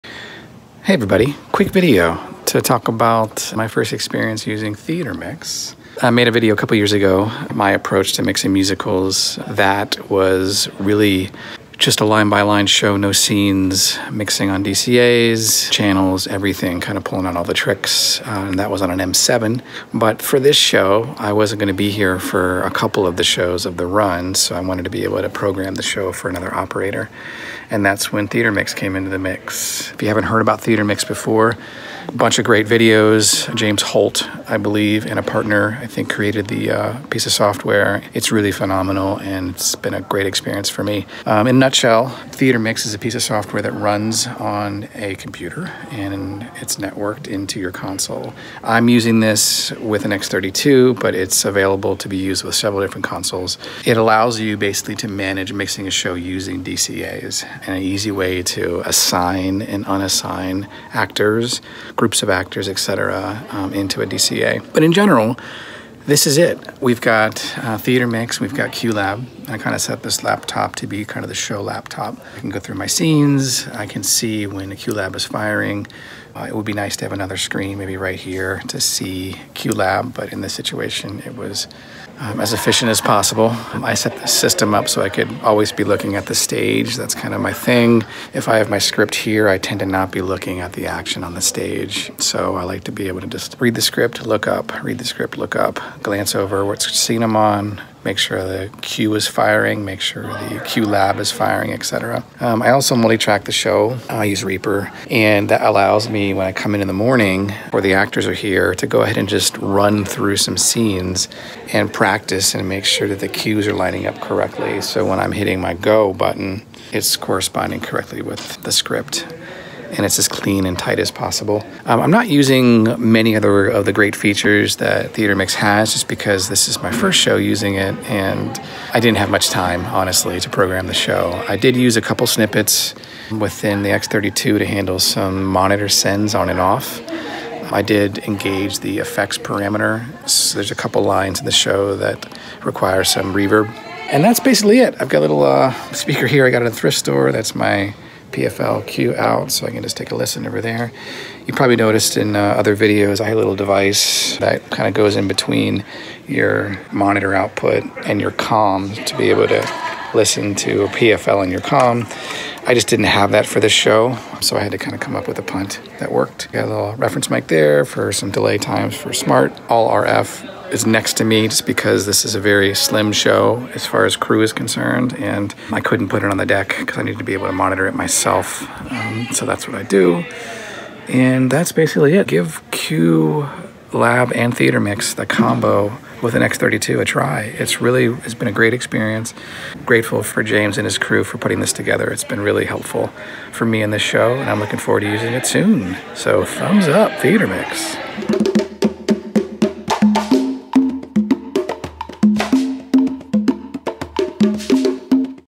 I used Theater Mix and loved it!
A quick video about my experience using Theater Mix software with QLab and a X32 while mixing a musical. TLDR(W): I love Theater Mix!

Thanks you @holty24 and  crew!

More live sound resources and help on my website!